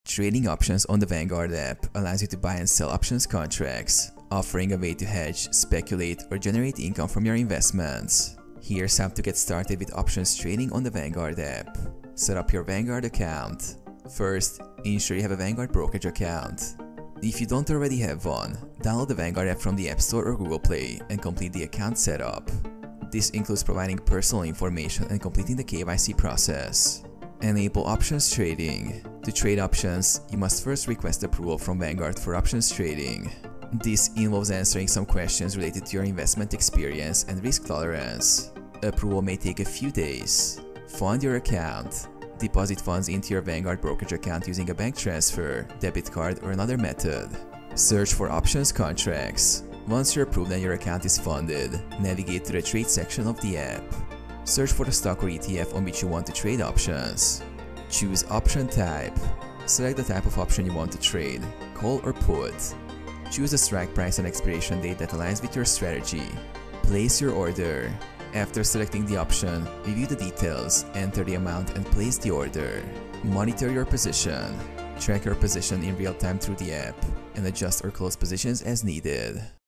How To Trade Options On Vanguard App (2025)
In this video I will explain How You Can Trade Options On Vanguard App In 2025.